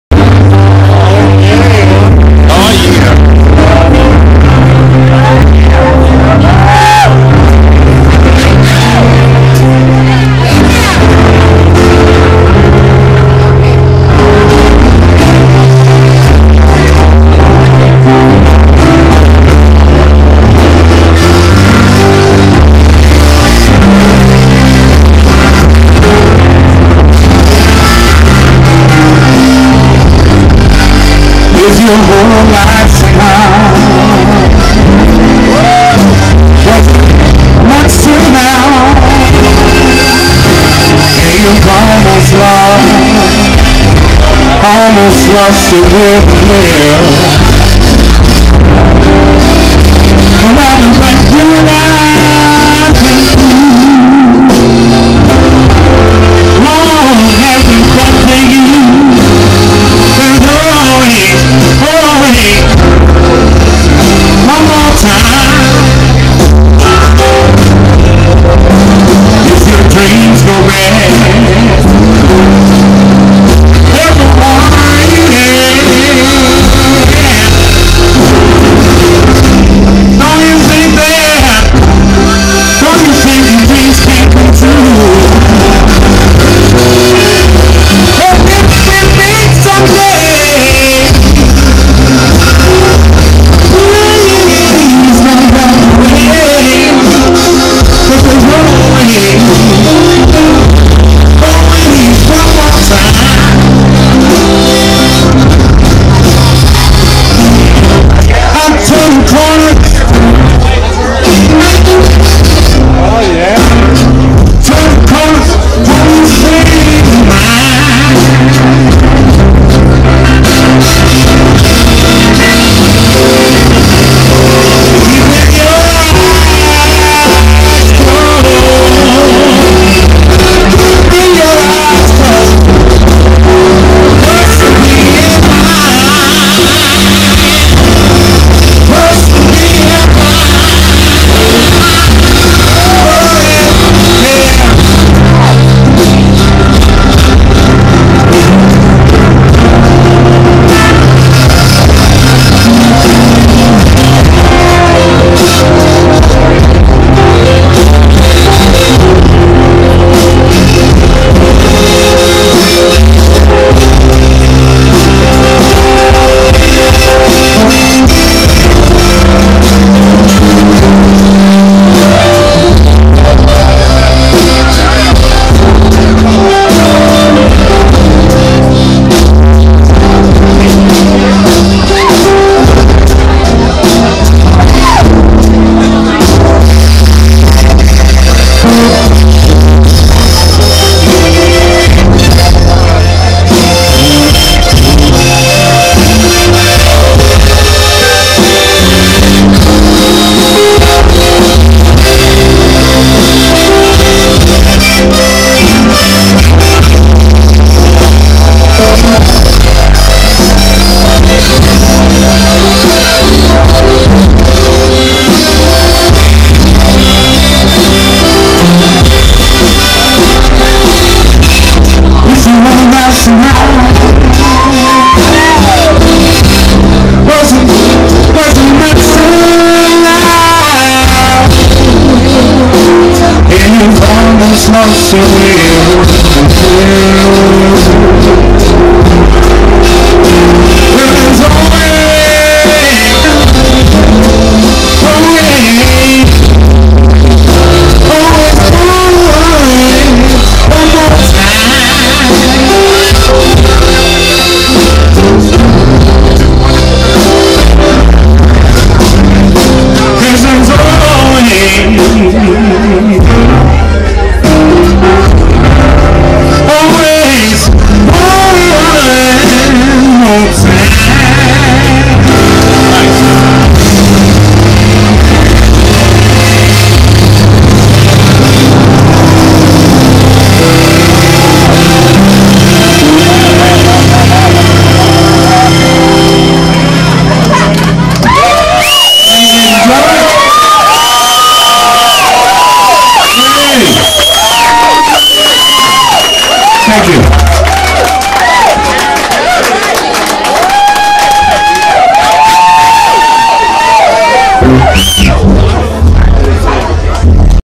Nick The Feelin Mrozinski at Live Schooner 2013-02-09 3/4 bad mic
Really awesome performances from Nick "The Feelin" Mrozinski at the Schooner last night but unfortunately my audio levels completely blew out. Still posting anyway b.c someone might still find a little enjoyment - there was great energy!!
Paul Manske memorial gig
CREATIVE COMMONS WITH ATTRIBUTION - PLZ REMIX